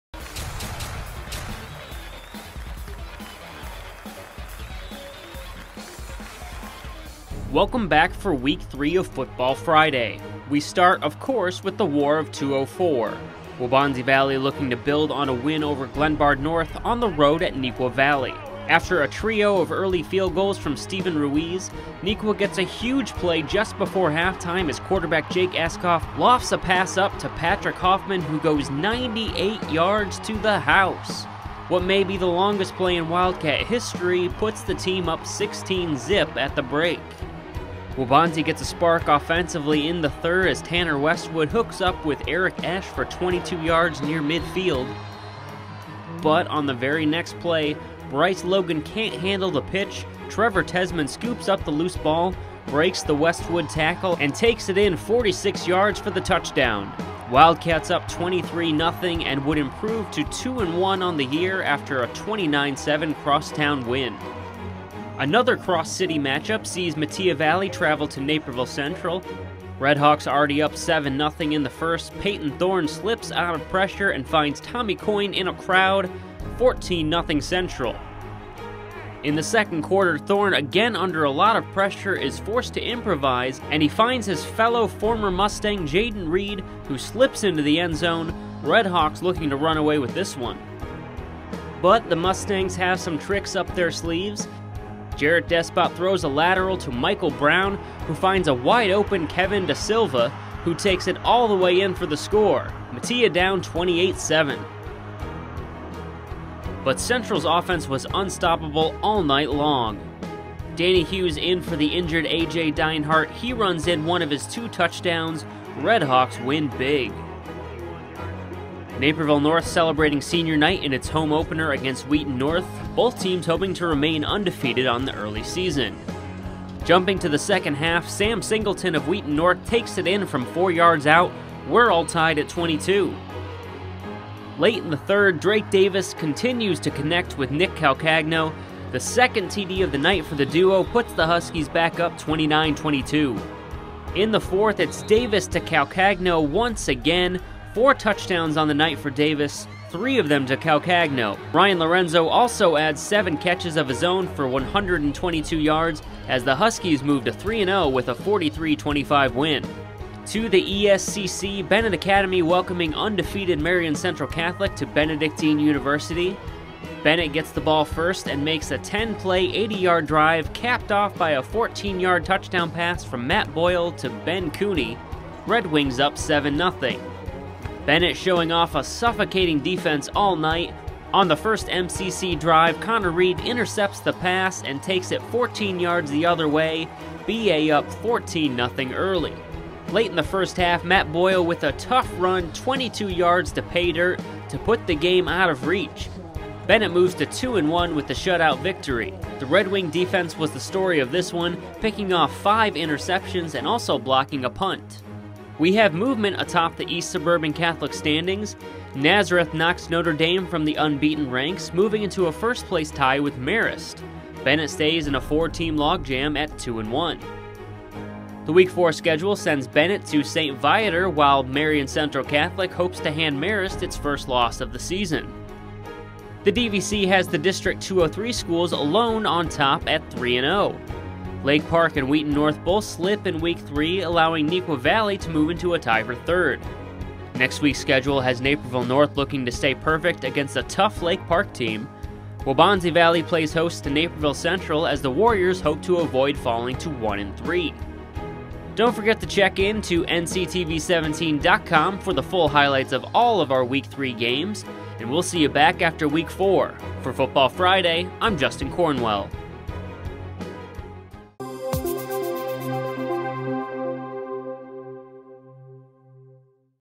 Football Friday 2017 // Week 3
Check out the Week 3 highlights of the Naperville high school football season!